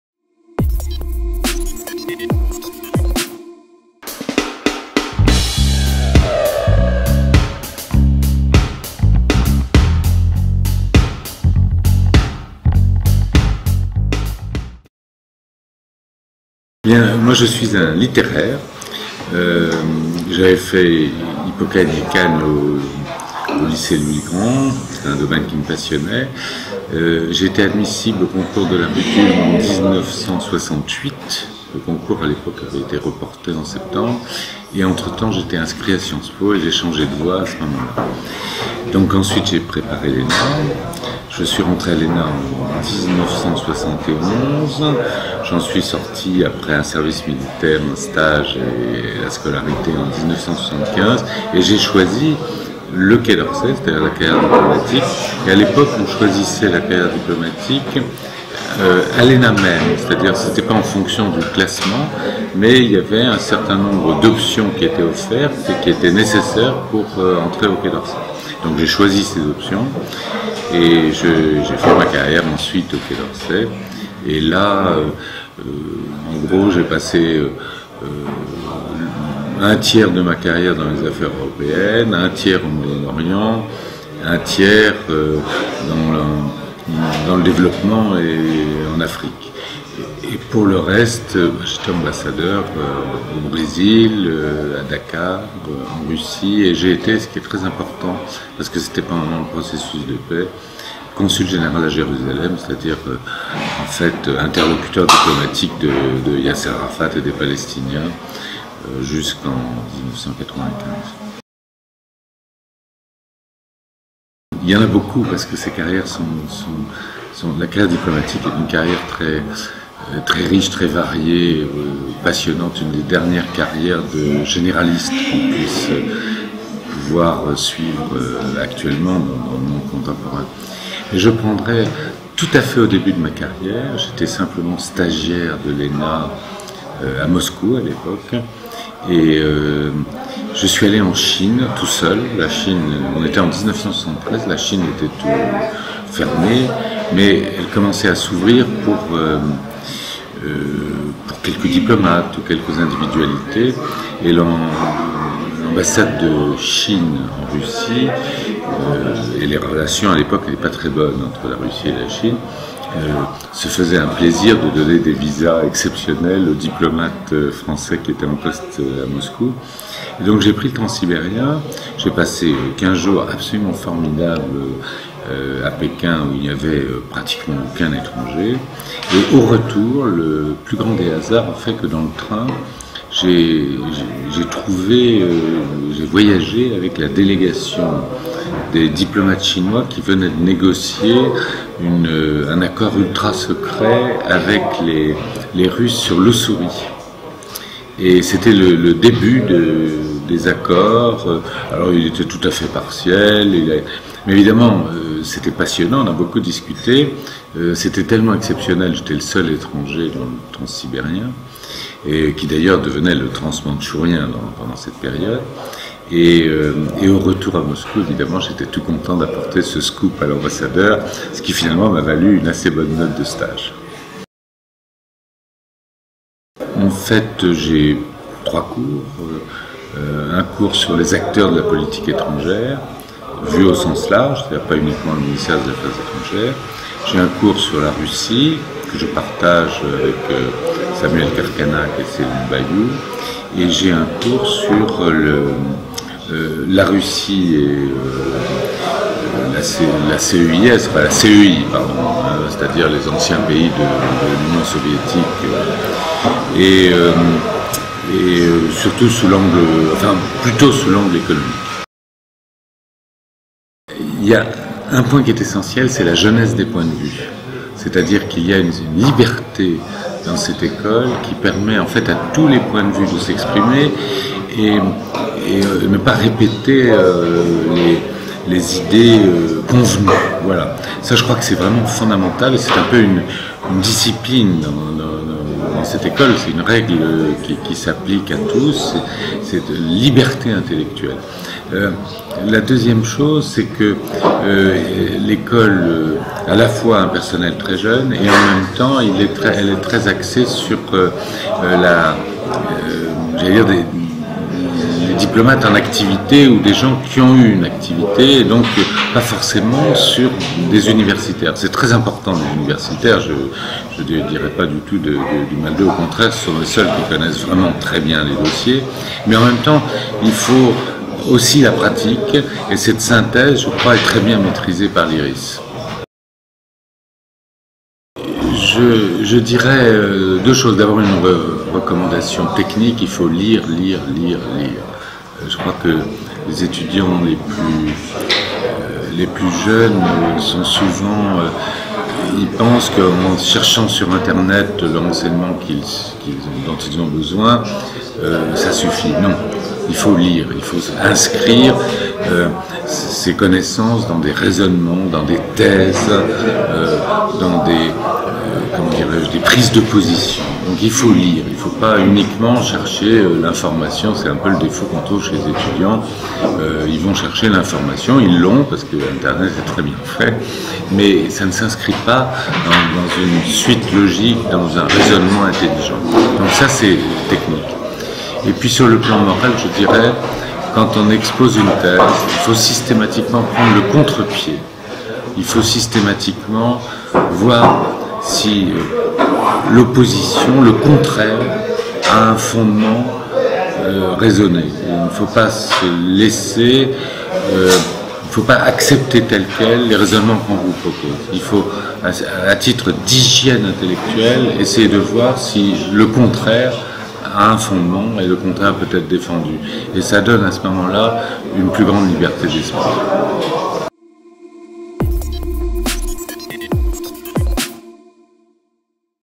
[Portrait d'enseignant] Rencontre avec Jean de Gliniasty
Jean de Gliniasty est directeur de recherche à l’IRIS, spécialiste des questions russes. Jean de Gliniasty a connu une longue carrière diplomatique (consul général de France à Jérusalem, ambassadeur de France au Sénégal, au Brésil et en Russie, etc.).

Rencontre avec un des enseignants d'IRIS Sup', l'école de l'IRIS.